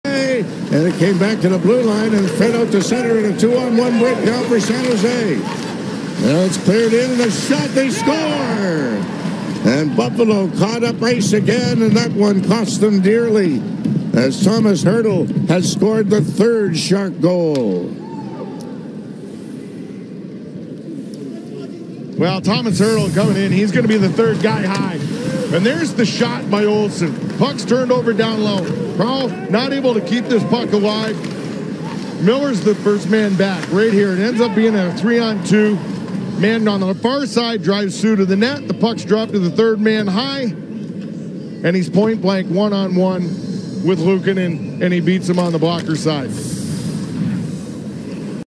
Tomas Hertl Makes It A 3-0 San Jose Lead After The First
The Sharks take a commanding lead heading into the first intermission as Tomas Hertl snipes this one top shelf on Ukko-Pekka Luukkonen